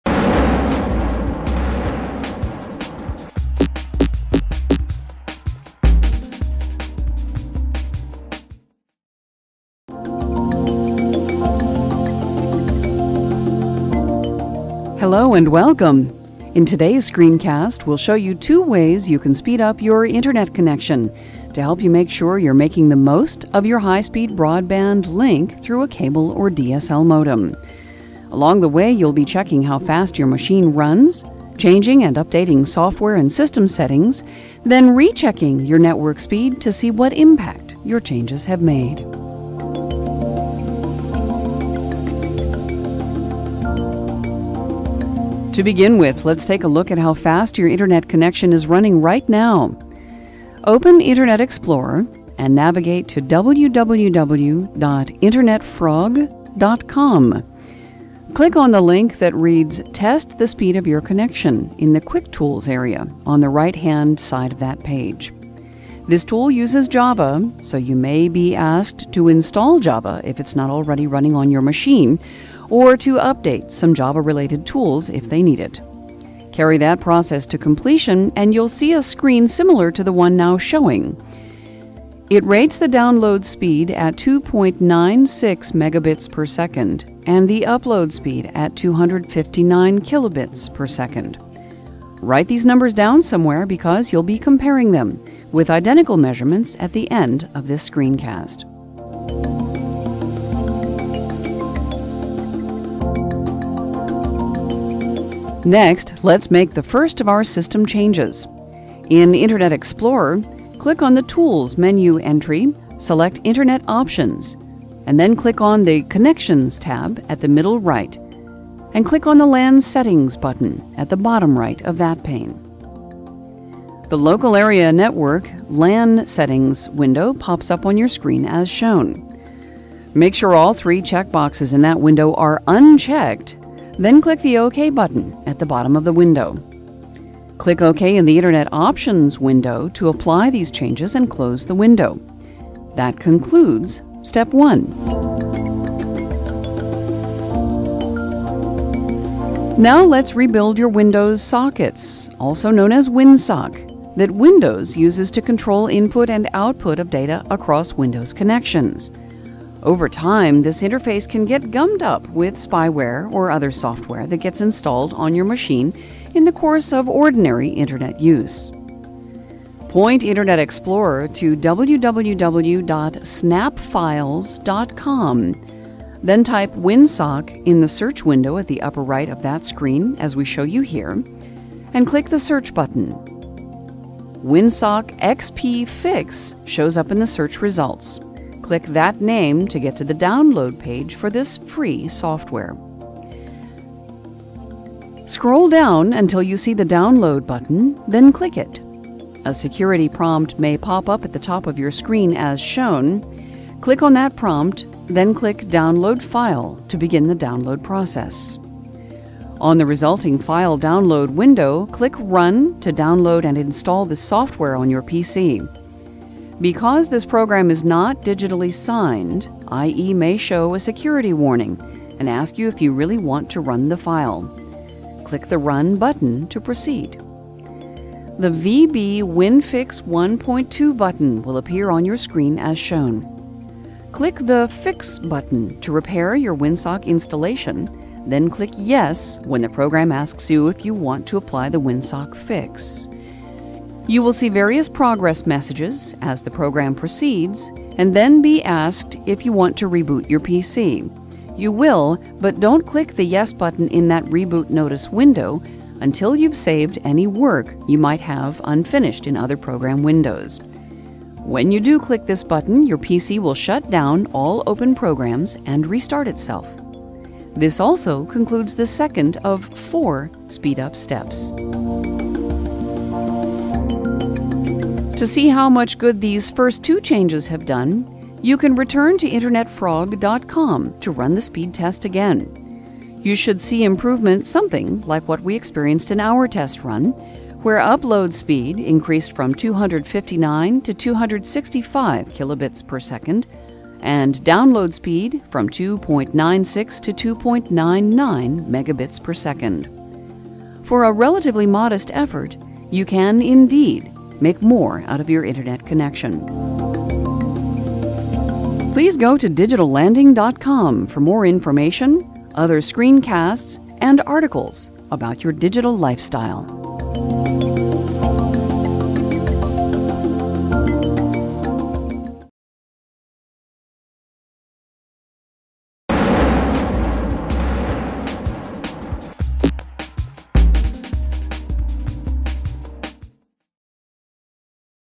How To Speed Up Your Internet Connection
Here are a few tips on how you can speed up your internet connection by changing a few simple Windows settings in this video from Digital Landing